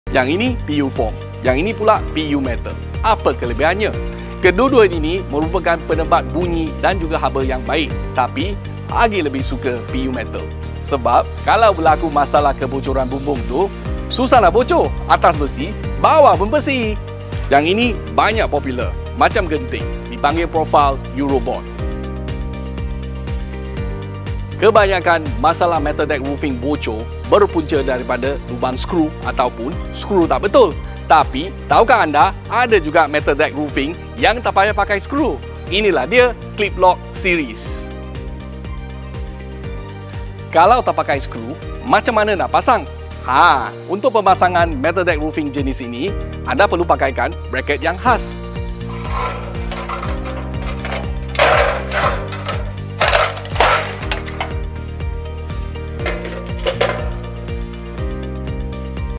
Jenis-jenis Metal Deck Roofing Yang Harus Anda Kenali - Part 4/5
Material
PPCR – Pre-Painted Cold Rolled Coil - Hoarding
PPGI – Pre-Painted Galvanised Coil - Roofing

Thickness/ Gauge
Example. G35, G32, G30, G28, G26
G35 = 0.21mm TCT
G30 = 0.30mm TCT
G24 = 0.47mm TCT

The higher the gauge number, the thinner the metal deck.
The lower the gauge number, the thicker or heavier the steel.
*TCT – Total Coated Thickness – measures the steel substrate as well as the metallic coating, resin coating and/or paint film.

Colour/ Warna
Prepainted - Standard Colour & Special Colour
No Colour - Aluminium Zinc/ Zinc Aluminium
Zinc – easier to shape, sturdy, but relatively low corrosion resistance, and prone to rust.
Aluminium – lightweight metal, flexible. Aluminium corrodes but it does not rust.
The combination of both aluminium & zinc – improve its strength, corrosion and heat resistance, and does not get rusty.

Profile/ Rekabentuk
Astino New Sunroof – Most popular profile, used for hoarding, fencing, cabin and roofing or walling for building.

Astino New Sunroof 3 in 1 – Combination of metal roofing, XLPE (Cross-linked Polyethylene) foam and aluminium foil. Provides good heat-resistant.

Astino New Sunroof PU Foam – A complete insulated roofing panel with outstanding thermal efficiency. Manufactured by bonding CFC-Free Rigid Polyurethane (PU) Foam between exterior profile metal roofing sheet and interior PVC lamination.

Astino New Sunroof PU Metal – A complete insulated roofing panel with outstanding thermal efficiency. Manufactured by bonding CFC-Free Rigid Polyurethane (PU) Foam between exterior profile metal roofing sheet and interior metal deck lamination.
• Excellent heat resistance, and good sound absorption.
• Reduce your building temperature with great savings on electricity.
• Good in preventing water leakage, due to its low water absorption.

Astino Uroll Bond – ideally suited as the best replacement for traditional roof tiles, that enhances the aesthetic appearance of any commercial and residential projects.

Astino Clip Lock Series – Premier profile that combines concealed fastening features with structural performance. No exposed fasteners are used thereby eliminating leakage occurrences and provide years of worry free performance.
• Guaranteed and reliable performance.
• Well suited for commercial, industrial and residential roofing applications.
• Fixing Clip/ Fixing Bracket

Accessories - Fasteners
Most Popular: Self Drilling Screws, for Wood & Metal structure.

- Malaysia's Largest Home Improvement Online Store.